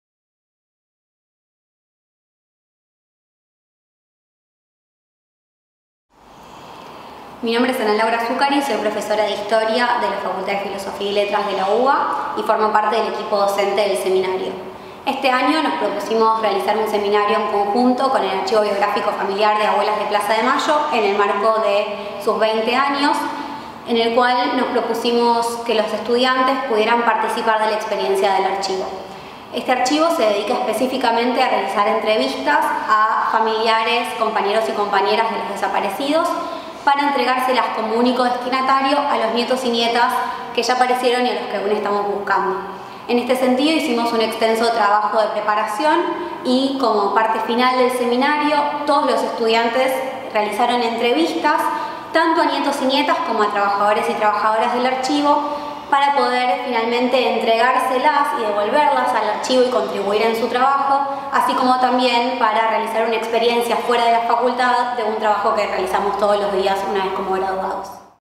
Cierre del seminario PST en el Archivo Biográfico Familiar de Abuelas
Las palabras de Ana Laura Sucari, profesora de historia e integrante de la Cátedra Libre de DD.HH.